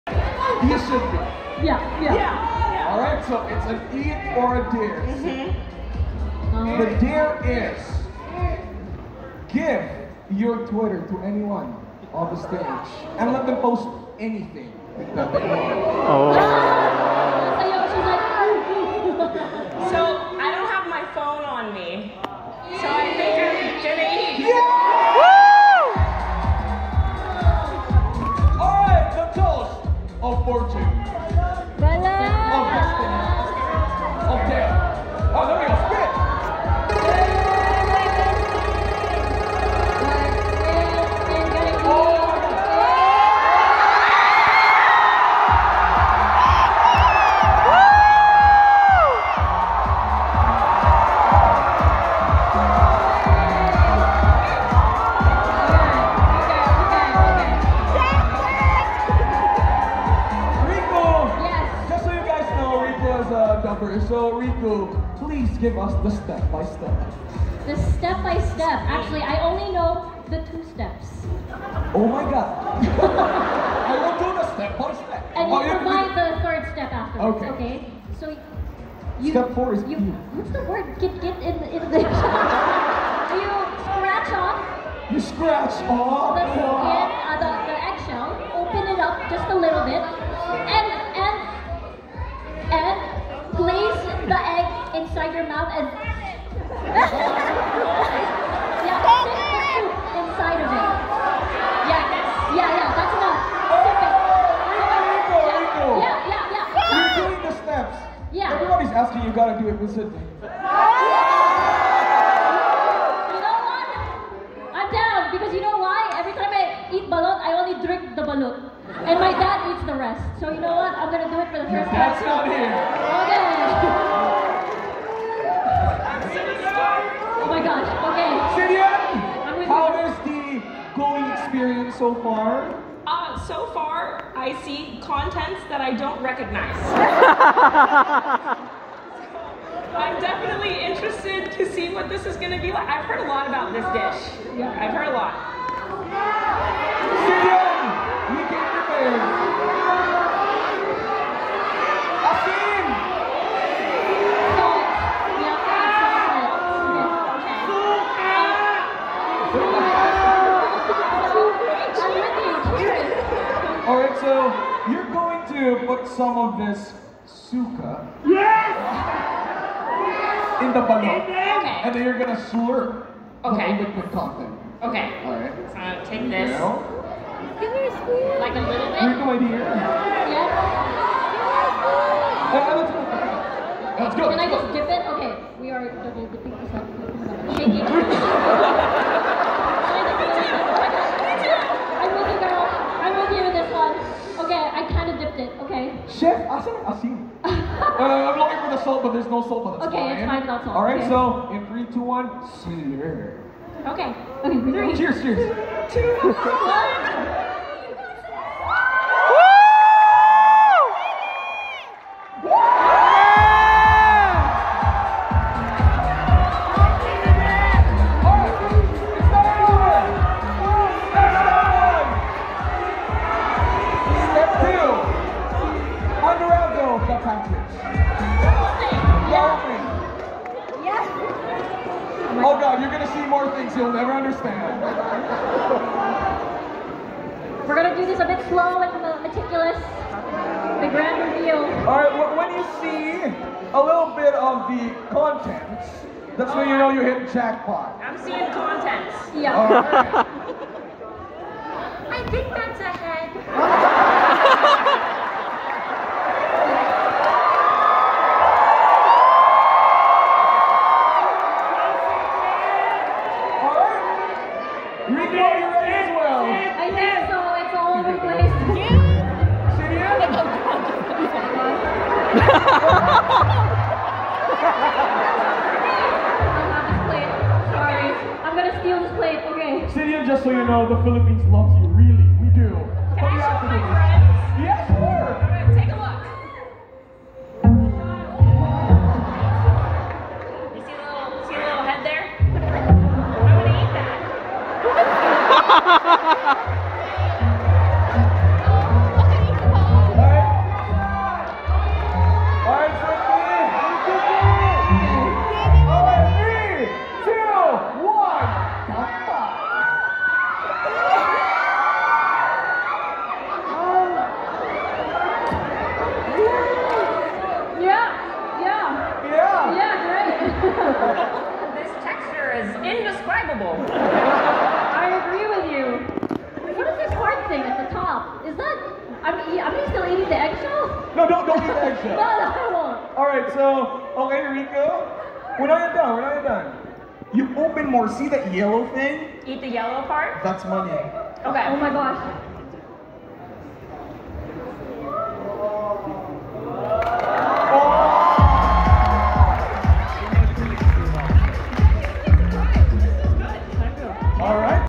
Sydeon tries balut for the first time - DSG Fan Meet Manila
You can do it, Syd. I believe in you.

Taken at the Disguised Fan Meetup Manila Event by Disguised Toast's DSG.